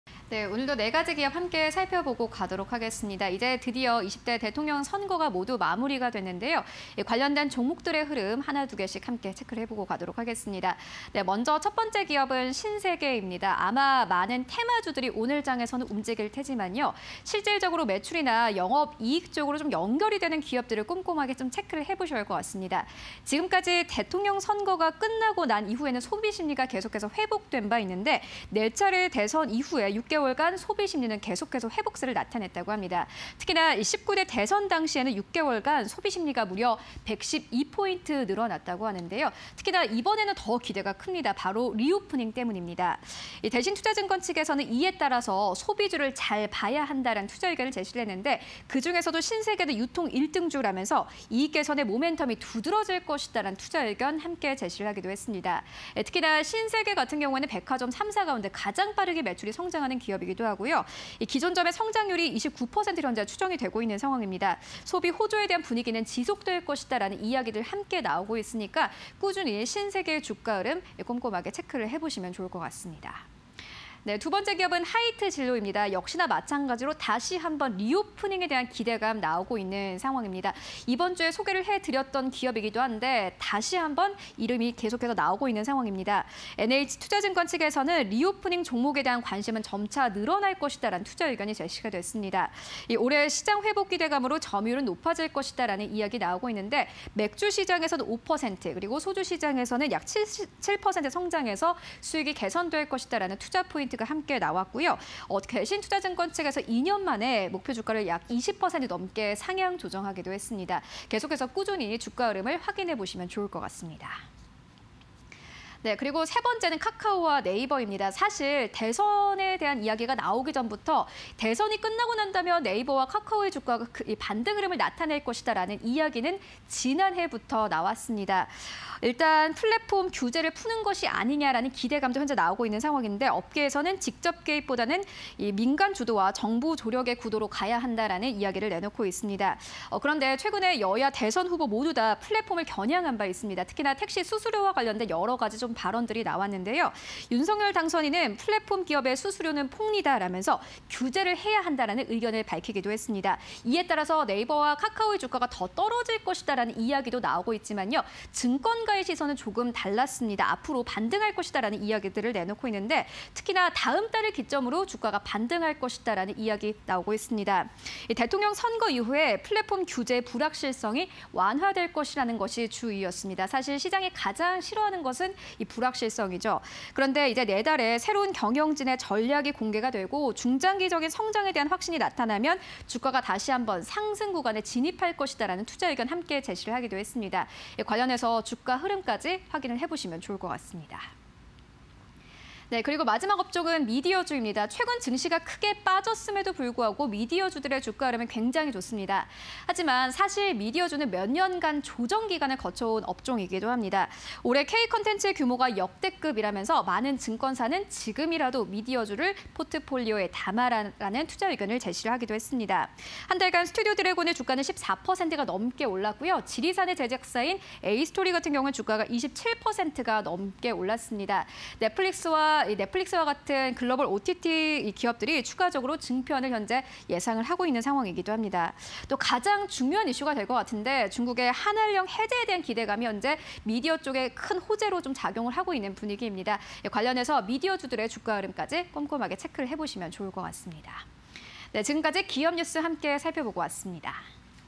신세계, 하이트진로, 카카오, 네이버, 미디어주_공시&뉴스 (20220310)
- 네 차례 대선 이후 6개월간 소비심리 회복
- 19대 대선 당시 6개월간 소비심리 112포인트 증가
- 대신證 “유통 1등주… 이익 개선 모멘텀 두드러질 것”
- 백화점 기존점 성장률 29%, 소비 호조 분위기 지속

- NH證 “리오프닝 관련 종목 관심 점차 확대”
- 올해 시장 회복 기대감에 점유율 높아질 것
- 대신證, 목표주가 20% 상향 조정

카카오·네이버
- 차기 정부, 플랫폼 규제 풀까… 기대감↑
- 업계 "직접 개입은 혁신성장 발목, 민간주도-정부조력 구도"
- 여야 대선후보 모두 플랫폼 겨냥한 바 있어
- 더 떨어질 것 VS 대선 후 반등할 것

- 올해 K 콘텐츠 규모 역대급
- 한 달간 스튜디오드래곤 14%·에이스토리 27%↑
- 넷플릭스 등 글로벌 OTT 추가 증편
- 中 한한령 해제 기대감 호재